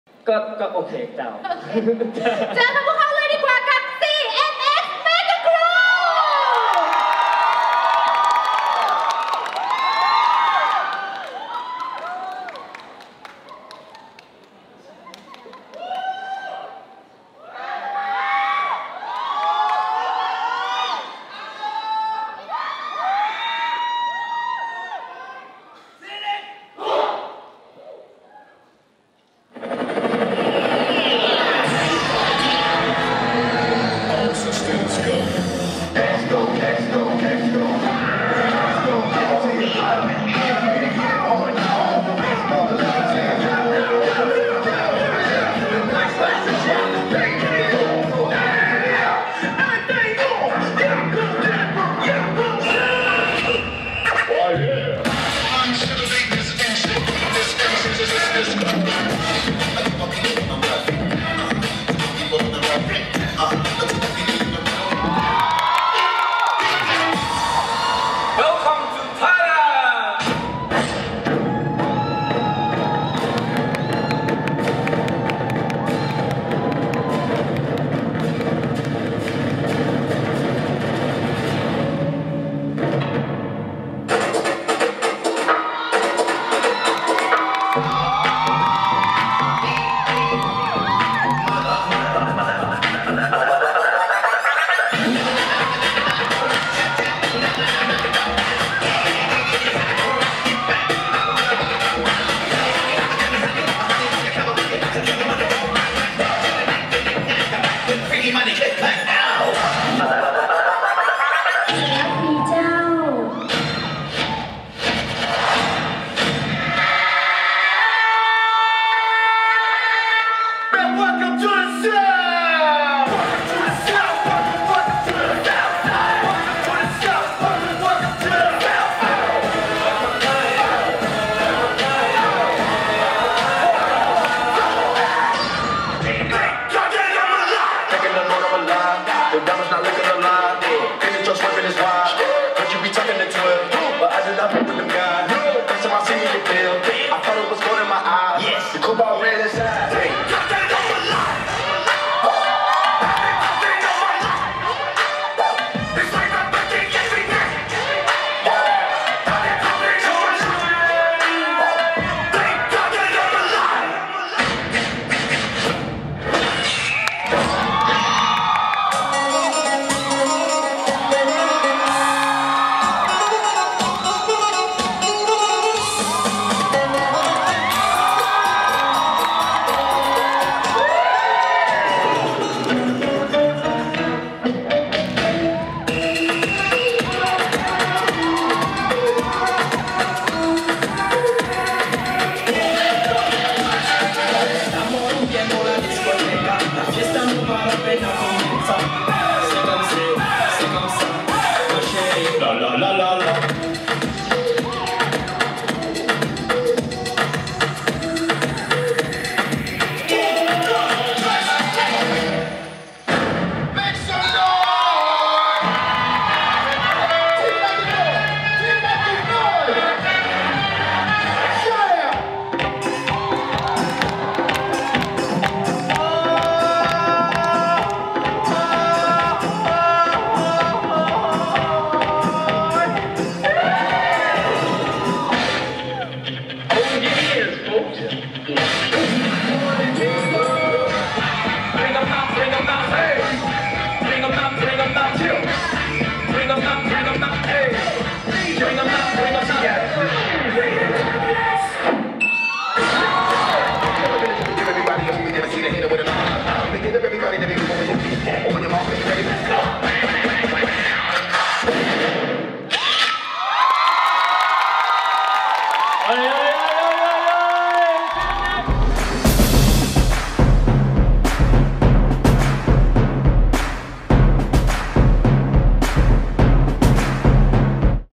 C.N.X Megacrew / Mega Crew Division / Finals at HHI THAILAND 2018
การเเข่งขัน Thailand Hiphop Dance Championship 2018 !!!
การแข่งขันที่เฟ้นหาสุดยอดทีมเต้น Hiphop ตัวแทนประเทศไทยเพื่อไปแข่งขันชิงแชมป์โลกที่ เมืองฟีนิกซ์ ประเทศสหรัฐอเมริกา ในรายการ World Hiphop Dance Championship 2018.